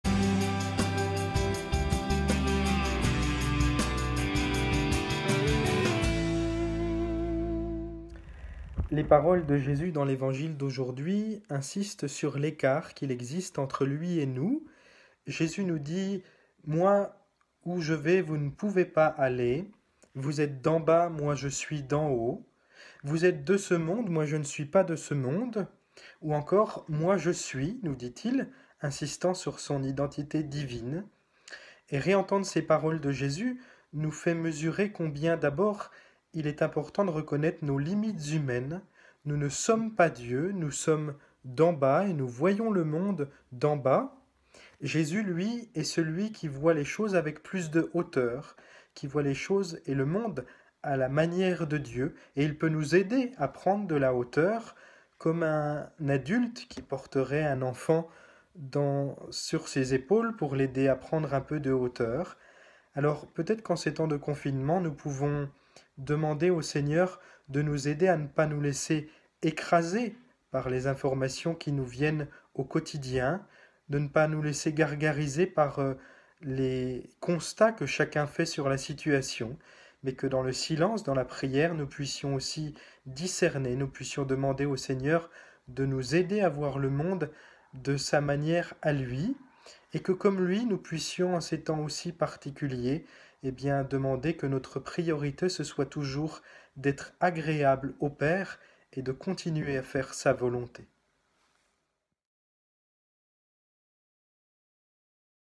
Confinés connectés n°17
[Confinésconnectés n°17] Aujourd’hui, prenons de la hauteur ! – avec le père Alexandre Voisard, curé de la paroisse Saint Jean Baptiste à Belfort.
Évangile du jour : « Quand vous aurez élevé le Fils de l’homme, alors vous comprendrez que moi, JE SUIS » (Jn 8, 21-30)